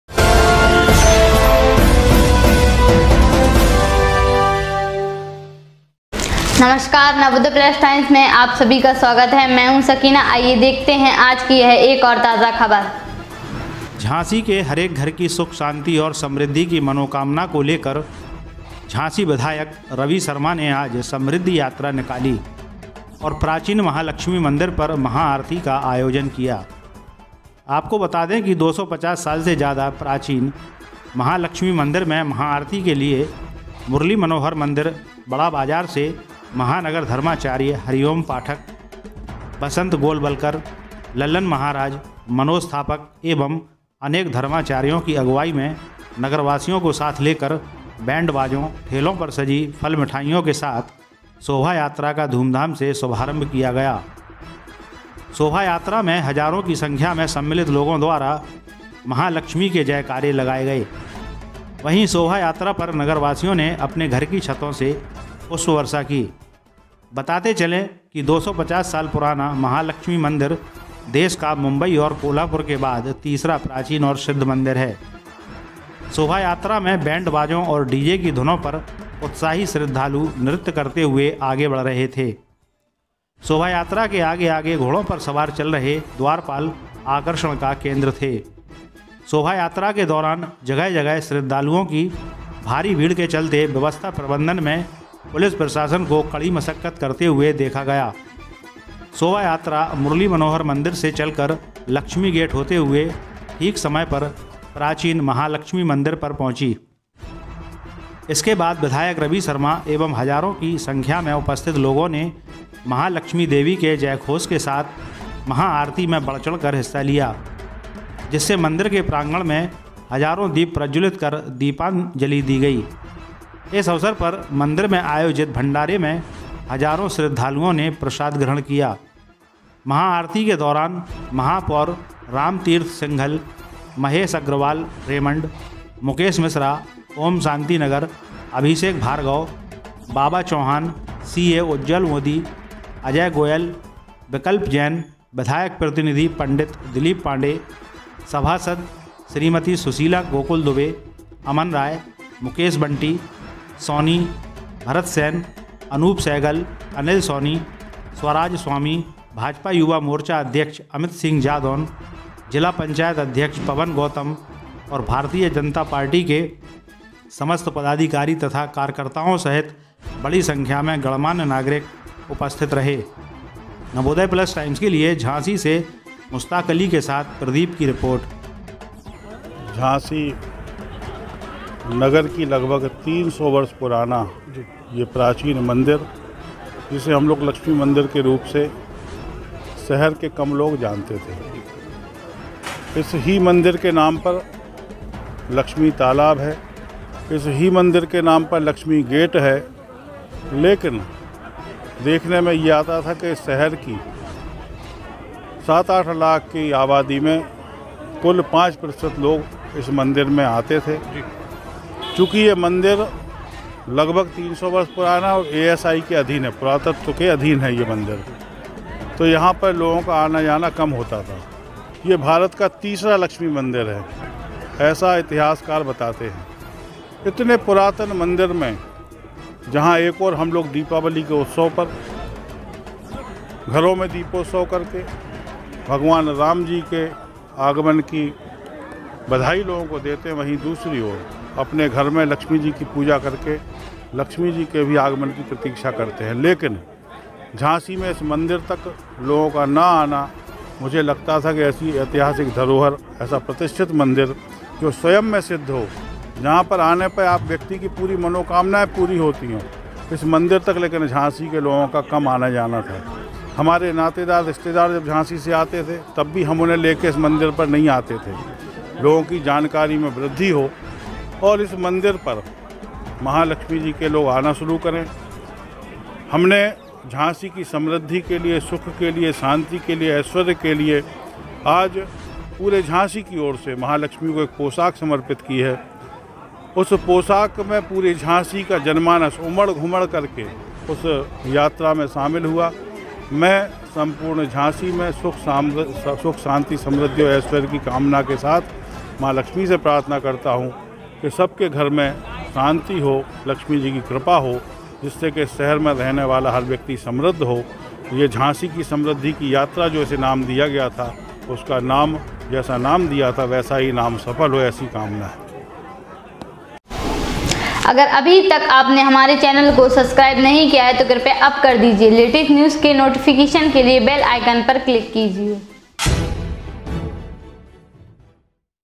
विधायक रवि शर्मा ने निकाली समृद्धि यात्रा, प्राचीन महालक्ष्मी मंदिर पर किया महाआरती का आयोजन।
रिपोर्ट : मुस्ताक अली, प्रदीप / झांसी